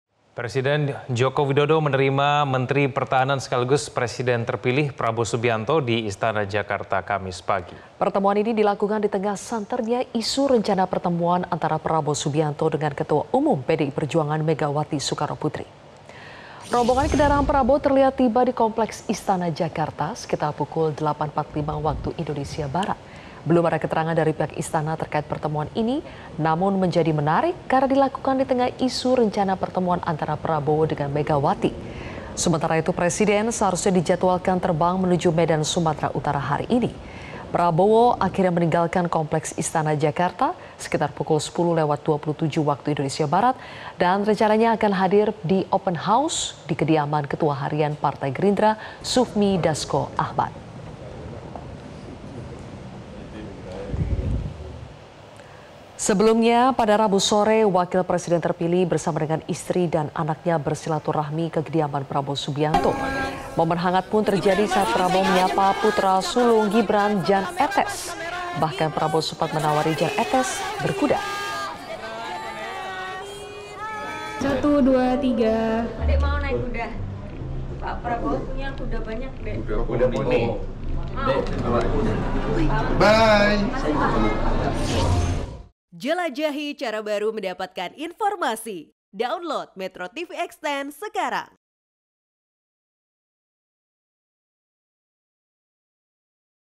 H +2 Lebaran, Prabowo Kembali Temui Jokowi
MetroTV, Presiden terpilih sekaligus Menteri Pertahanan Prabowo Subianto kembali menyambangi Presiden Joko Widodo (Jokowi) di Istana Kepresidenan pada hari kedua Idul Fitri 1445 H, Kamis (11/4/2024).